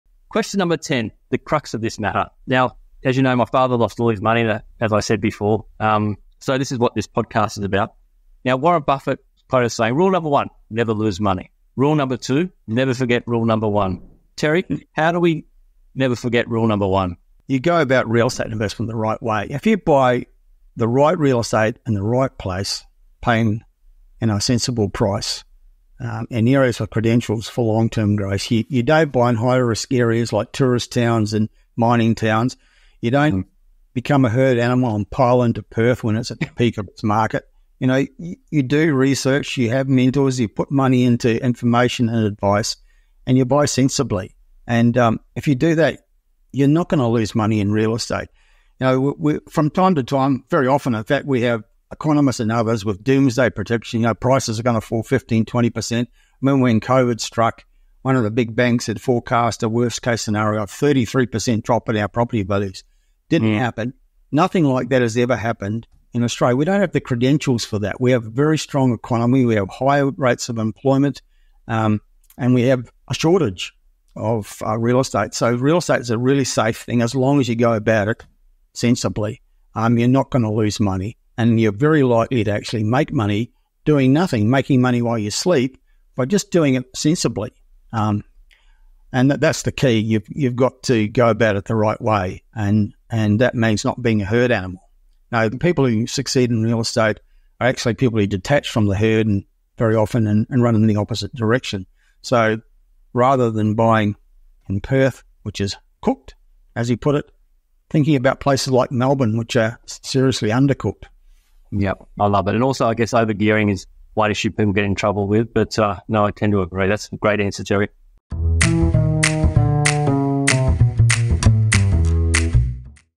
Cracking the Code: Real Estate Strategies from Terry Ryder
Join me and Terry Ryder CEO of Hotspotting as we delve into our personal definition of success, ranging from building a thriving business to finding balance in an idyllic hometown environment.

Don't miss out on this episode brimming with practical advice and heartfelt reflections. Tune in now to discover our secrets to success and learn how you can apply them to your own life journey.